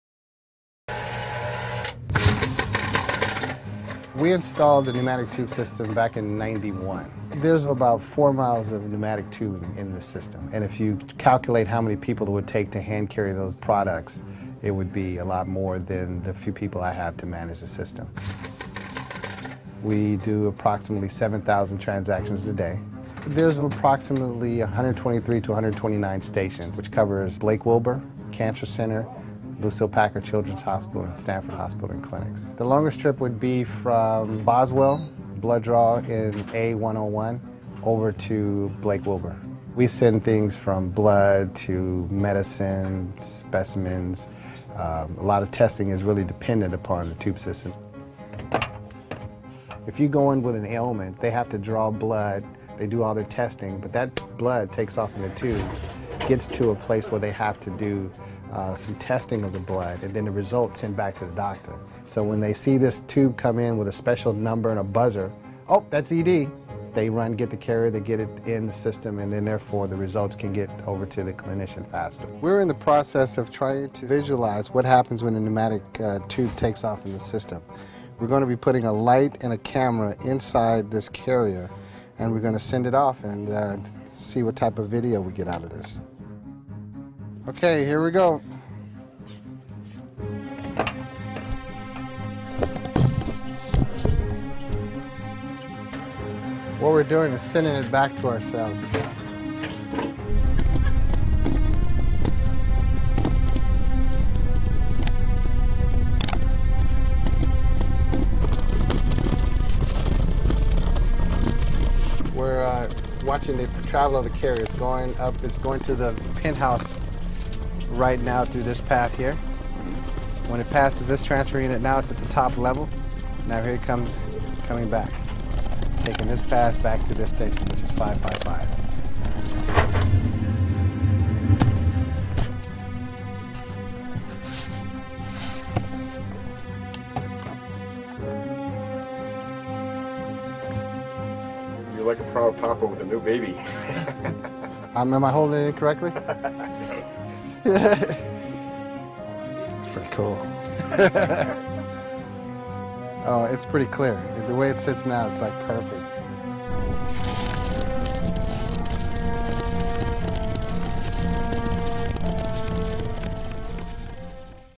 Peering inside a pneumatic tube network
Sometimes digital communication just can't cut it: time to bring the pneumatic tube networks out of retirement